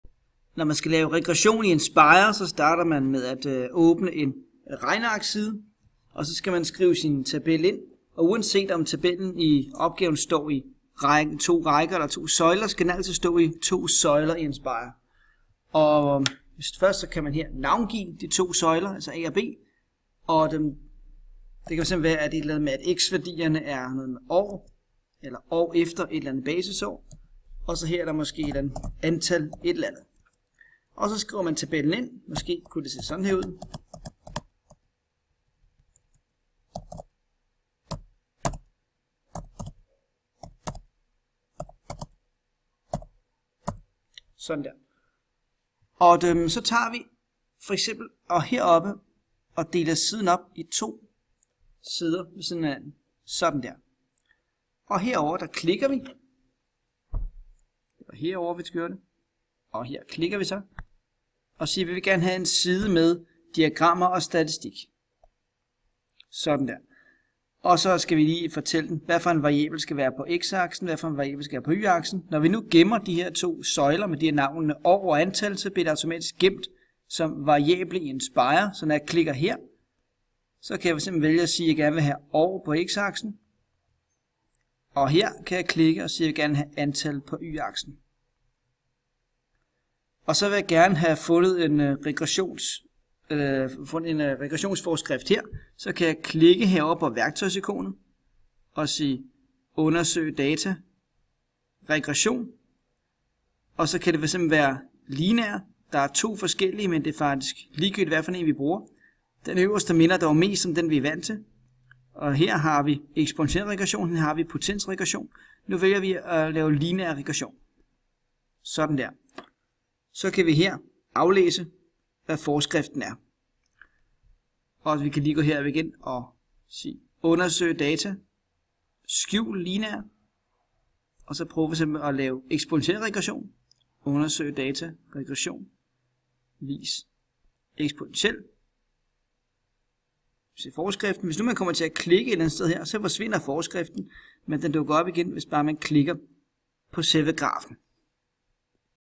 Regression i nspire
Hvordan man laver regression på et datasæt i Nspire